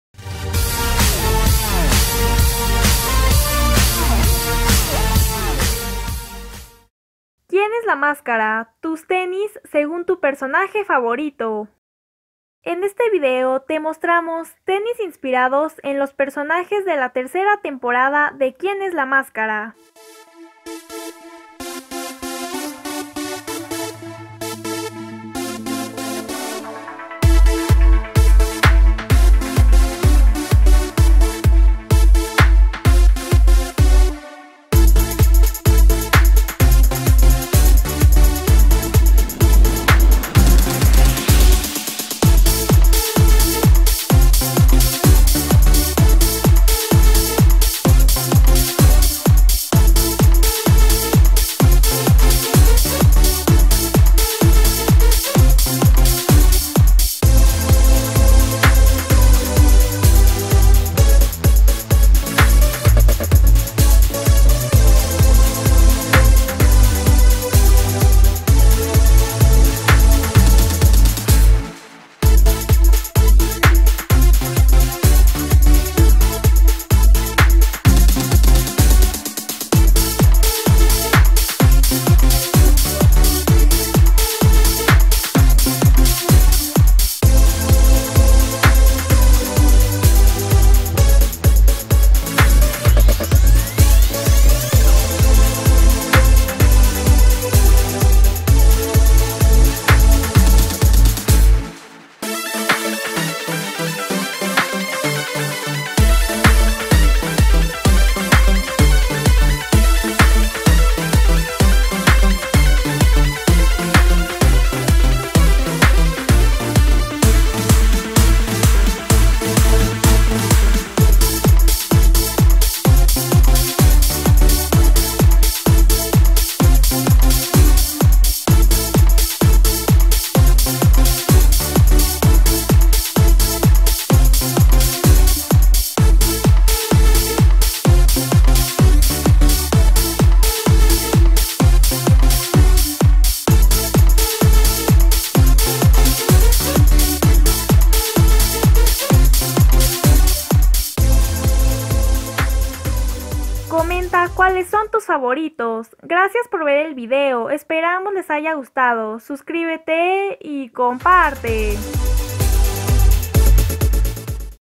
¿QUIEN ES LA MASCARA? - TUS TENIS SEGUN TU PERSONAJE FAVORITO | 3a TEMPORADA
En este video te vamos a mostrar tenis inspirados en los personajes de la tercera temporada de quien es la mascara.

quien es la mascara programa, presentaciones quien es la mascara,